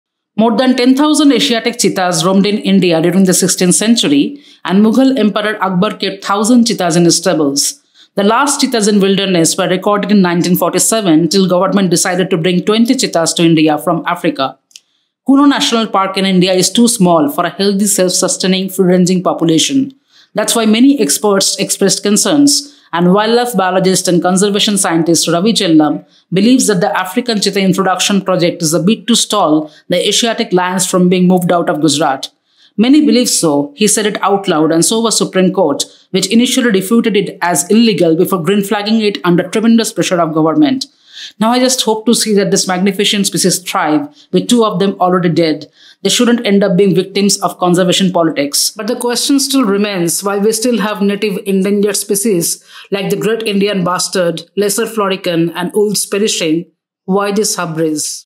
Introducing African Cheetahs to Kuno National Park of India is a good idea?
Indian wildlife conservation is anxiously waiting to see how introduction of Cheetahs in Kuno National Park works out. India does not have the habitat or prey species for wild, free-roaming African cheetahs, experts say. The calculated carrying capacity of three cheetahs per 100sqkm for Kuno was based on an out-of-date density estimate from Namibia.